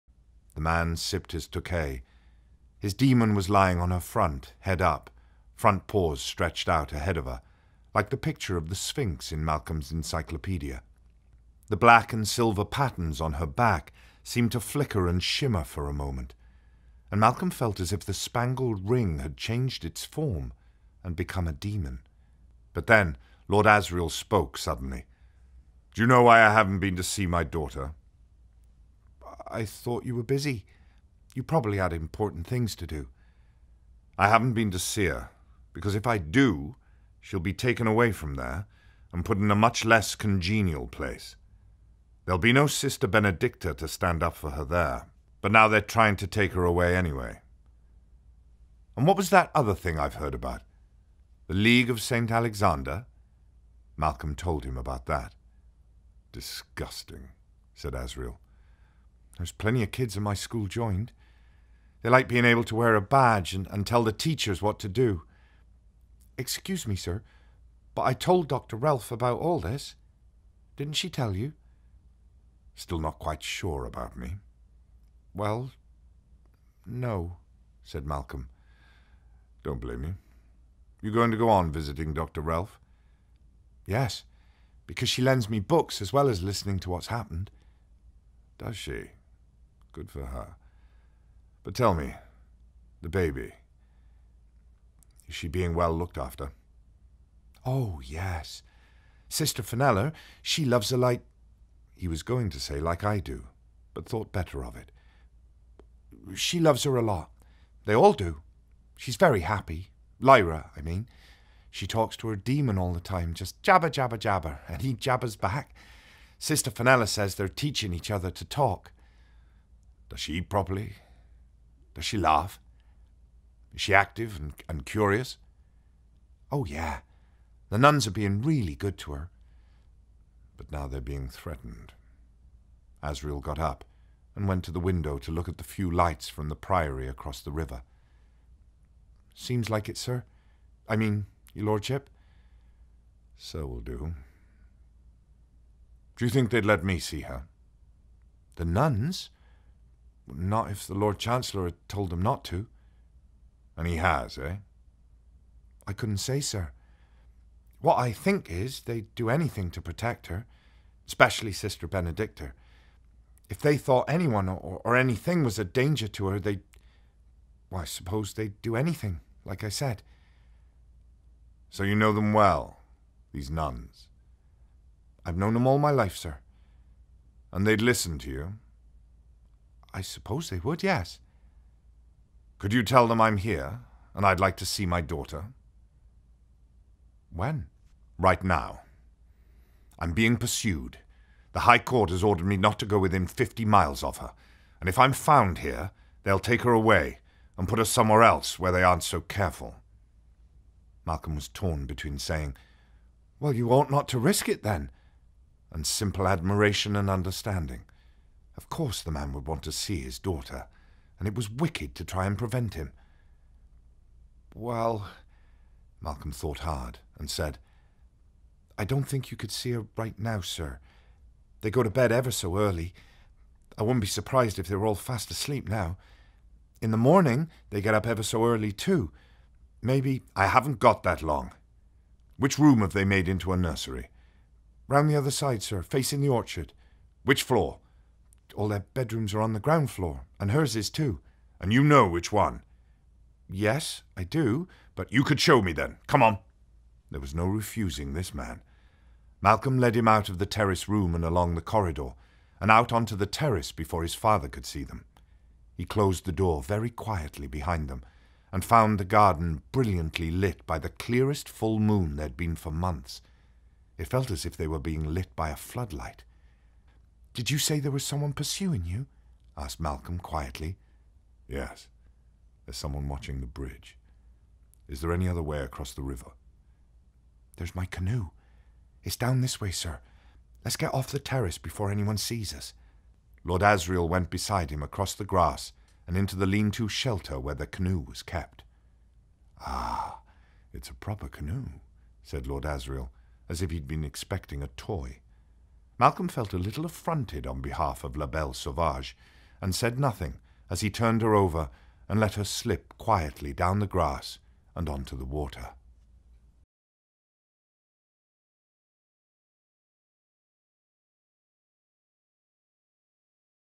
Michael Sheen Narrates The Book of Dust: La Belle Sauvage by Philip Pullman
Listen in as award-winning actor Michael Sheen (Passengers, Frost/Nixon, The Twilight Saga) narrates the audiobook of Philip Pullman’s "The Book of Dust Volume One: La Belle Sauvage." The audiobook will be published by Listening Library on 10/19/17.

One of the most highly-anticipated publications of 2017, "La Belle Sauvage" will return to the world of "His Dark Materials." It opens 10 years before Northern Lights and introduces a new hero: eleven-year-old Malcolm Polstead and his dæmon, Asta, who live across the river from Godstow Priory, where the nuns are sheltering a special guest– a baby by the name of Lyra Belacqua.